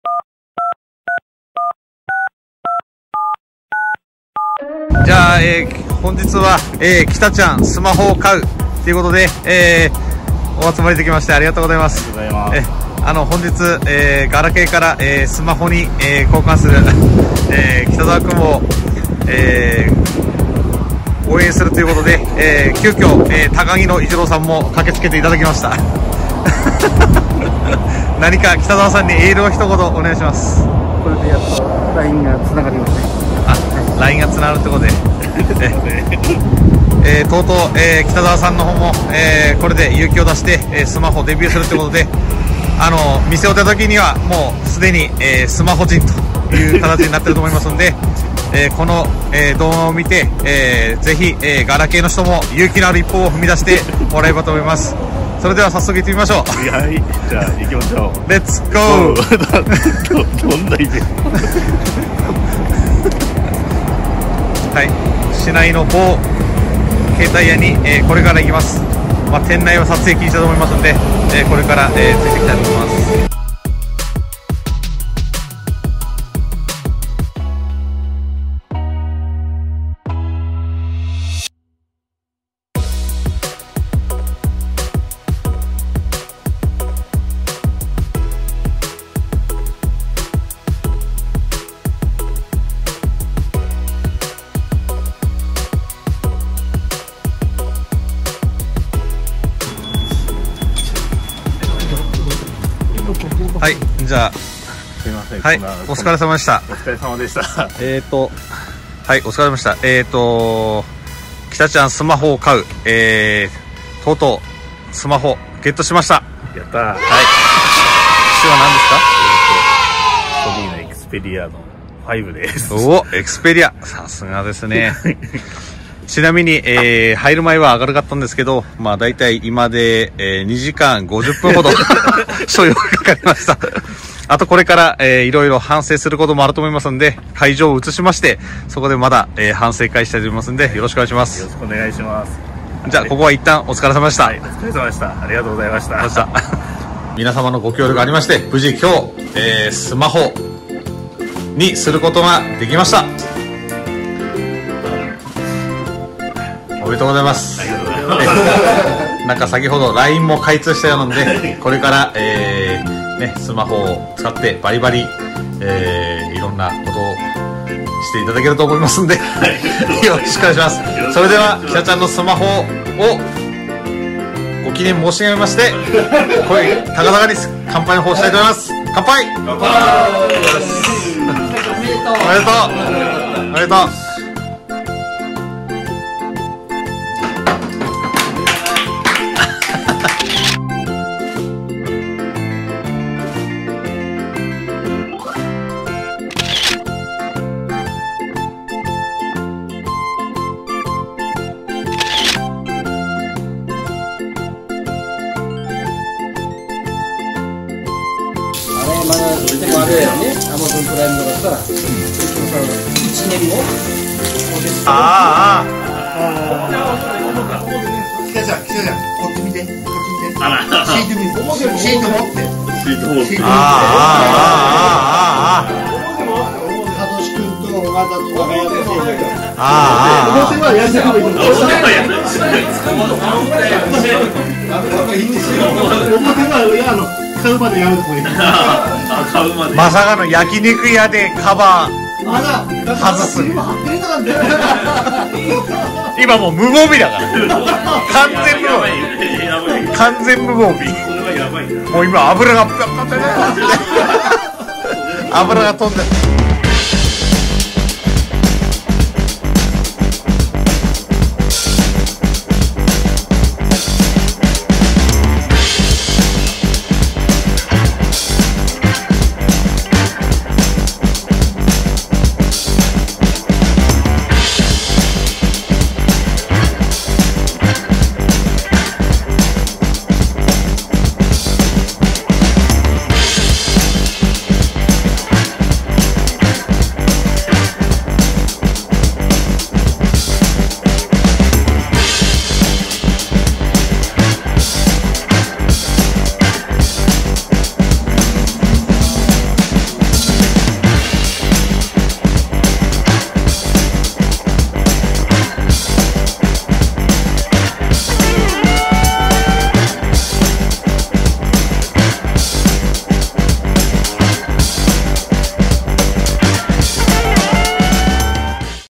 【スマホ初心者必見！】北ちゃんスマホを買う！
11年お世話になったガラケーに別れを告げ、勇気を出してスマホを買う北ちゃんの模様をお届けします！